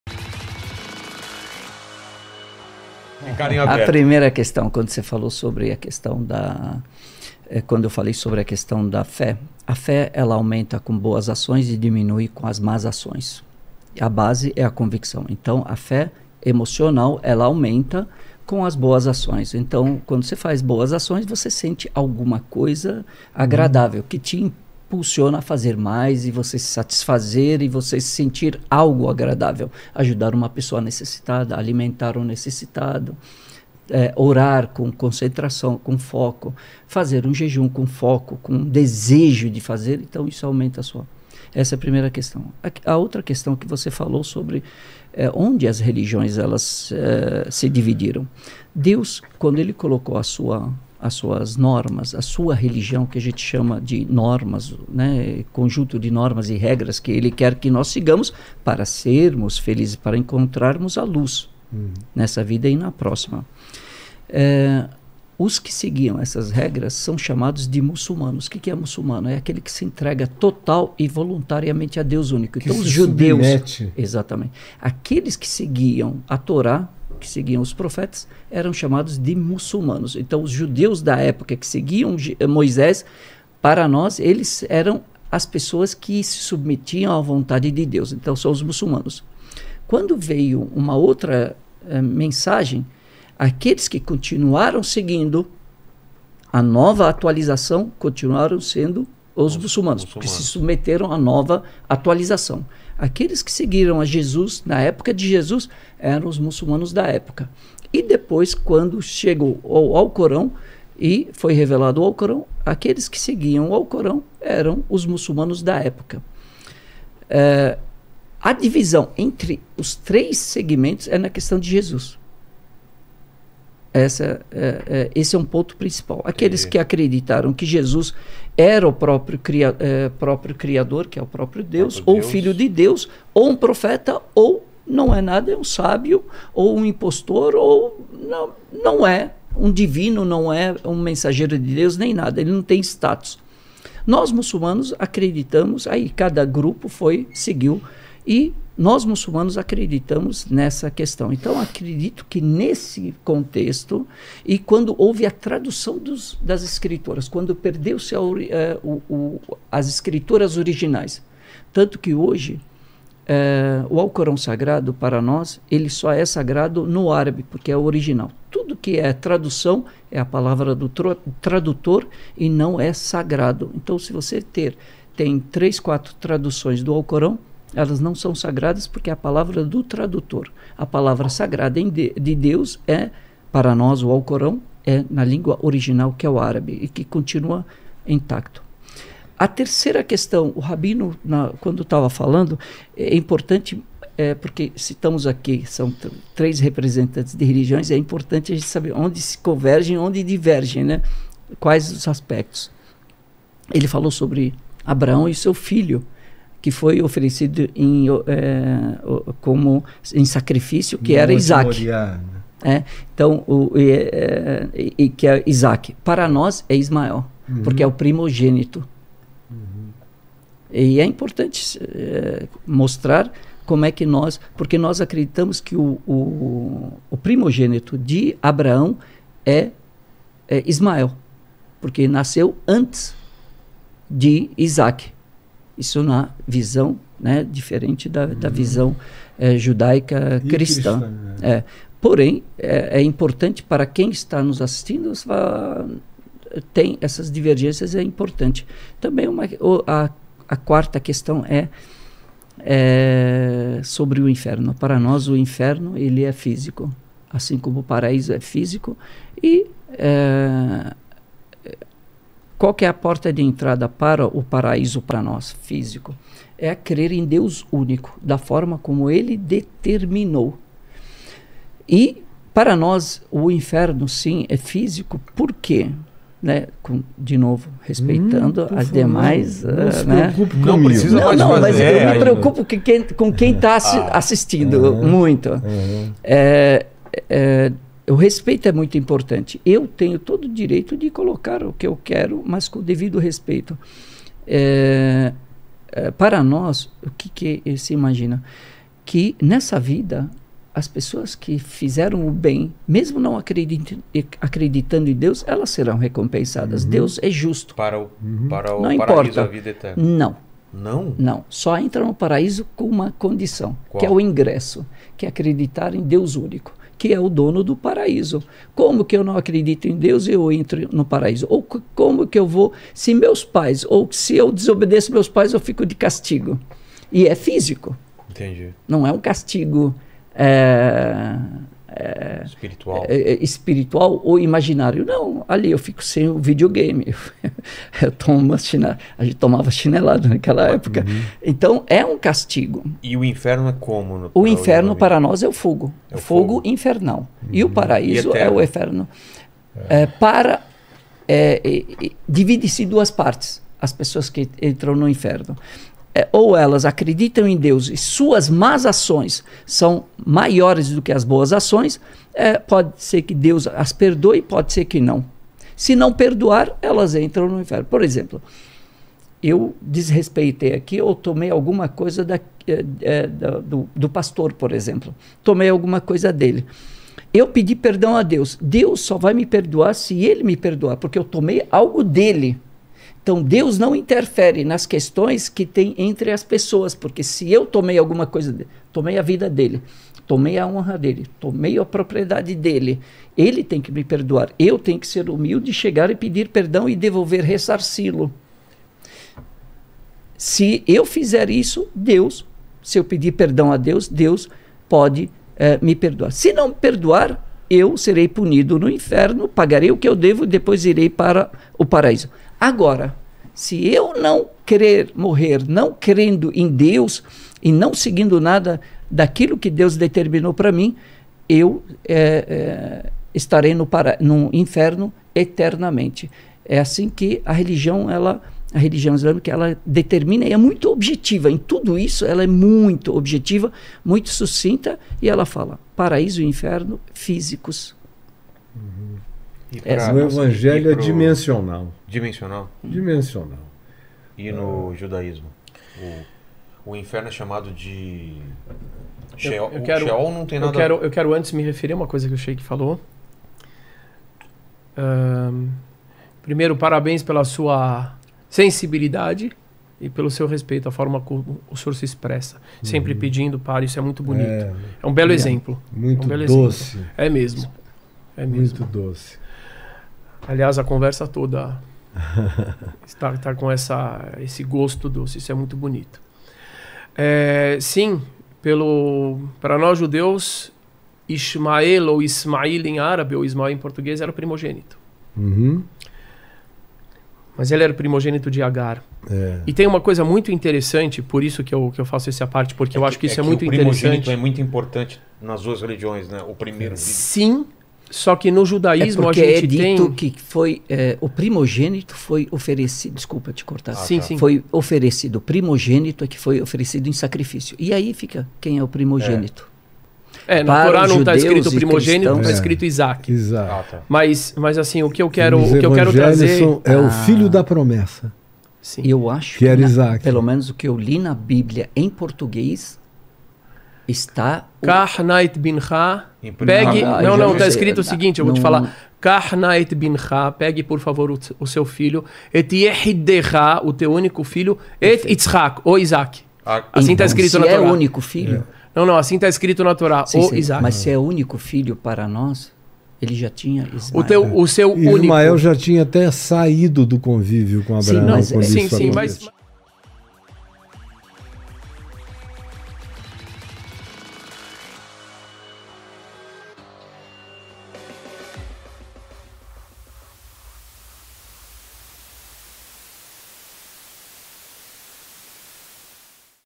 COMO o ISLAM vê a CRENÇA de OUTRAS RELIGIÕES? - CAIO FÁBIO, SHEIKH JIHAD E RABINO VENTURA
CAIO FÁBIO é cristão, SHEIKH JIHAD é muçulmano e RABINO VENTURA é judeu. O Vilela convidou os três só pra poder fazer a piada “um pastor, um sheik e um rabino entram no meu estúdio…”.

CAIXA POSTAL
Caixa Postal: 81969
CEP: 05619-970

EDIÇÃO:  Isabelly, Yasmin e Gabriel
PRODUÇÃO: Rogério Vilela
PAUTA E PESQUISA: Fabio Mantoanelli